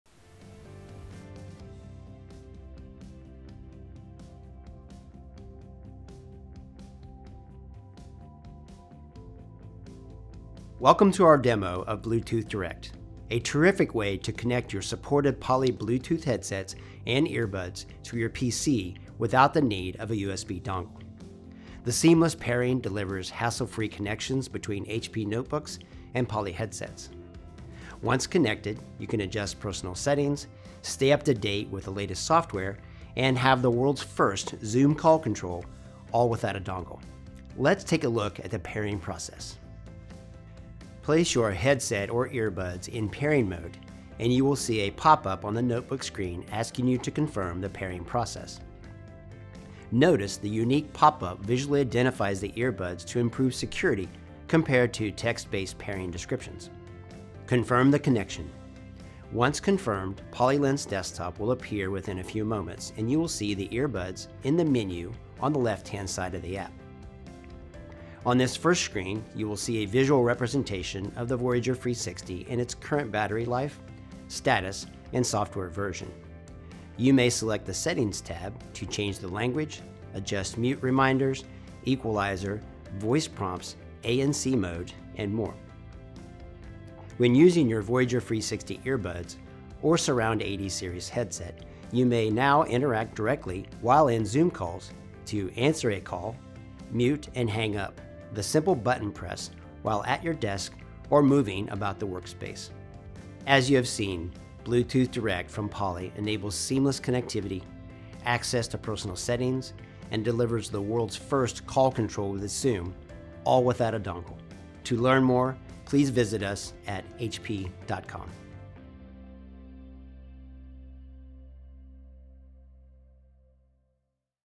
Poly Bluetooth® direct
HP | Poly now enables the world's first Bluetooth® headsets that deliver call control for Zoom without a USB dongle. The new feature accompanies an exclusive HP Differentiated Pairing experience with a single click pairing on HP PCs with Voyager Free 60 UC Series and Voyager Surround UC Series.

These features and more are available on Poly Lens Desktop version 2.0 for Microsoft® Windows. Watch here to see a demo.